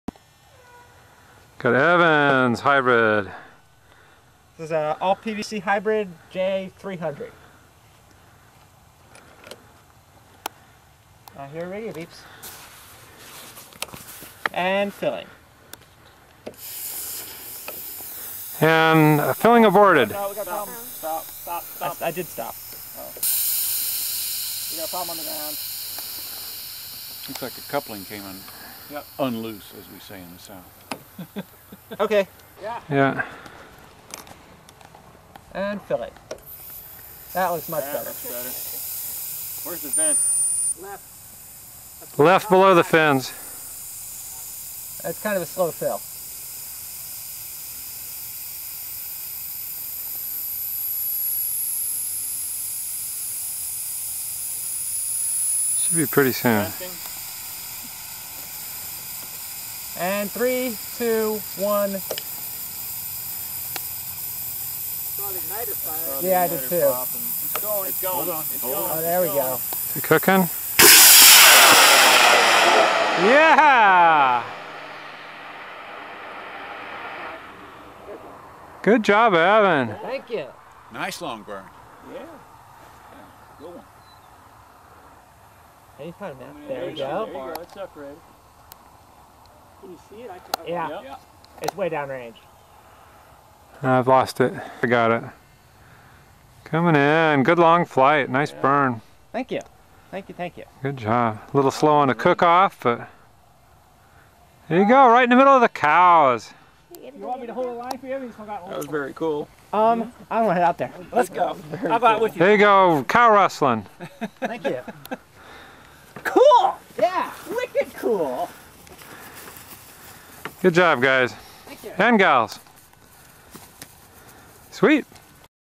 Blue Moon launch
The Blue Moon launches on an experimental hybrid motor.  Both rocket and motor were built by Evan Daniel.  Best estimate is that altitude was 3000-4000 feet on a J class motor, but the data was lost due to electronics failure.

Mike Harris
rocket & motor construction: Evan Daniel
cameraman: Mike Harris